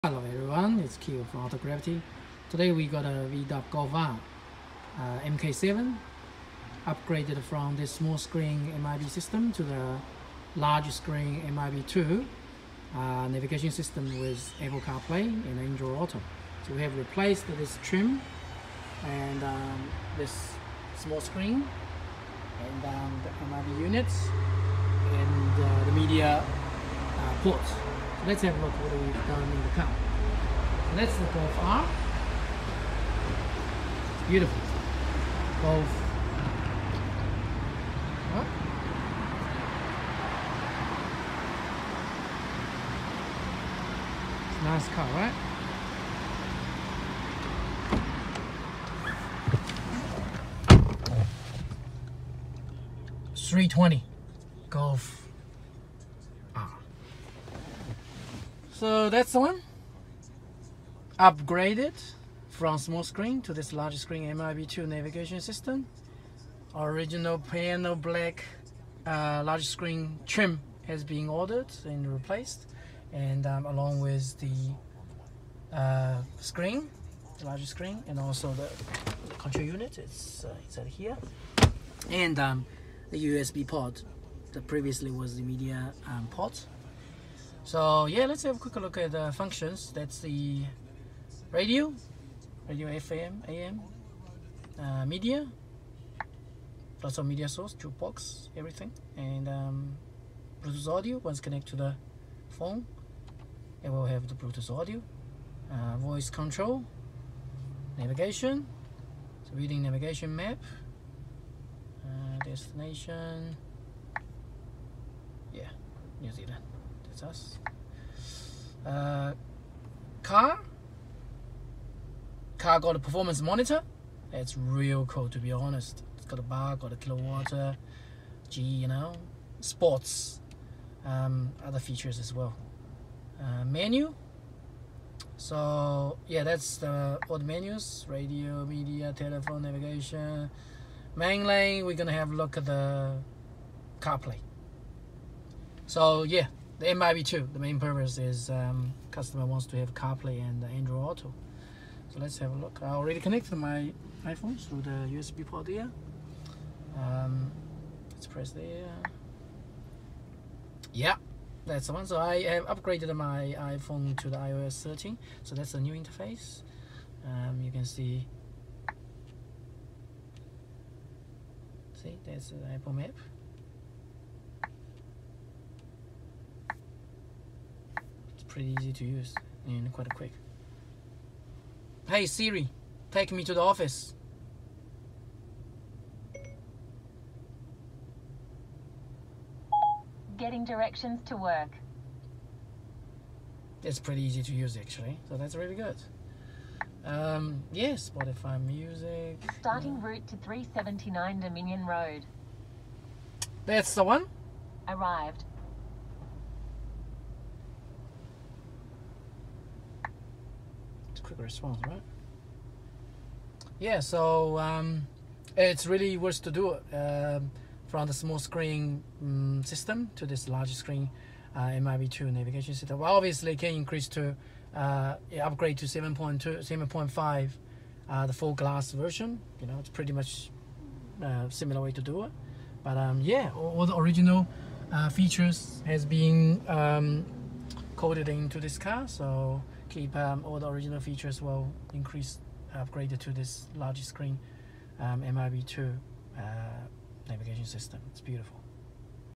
VW Golf R MK7 MIB2 discovery pro retrofit with CarPlay
VW golf R upgrade from small screen MIB SD map to discovery pro MIB2 navigation system with Apple CarPlay and Android Auto.